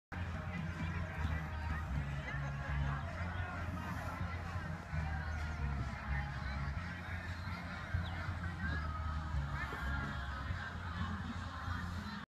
Shawnee inn at night 5/31/2014
Just had a nice meal at River Room in Shawnee Inn...now enjoying a bench!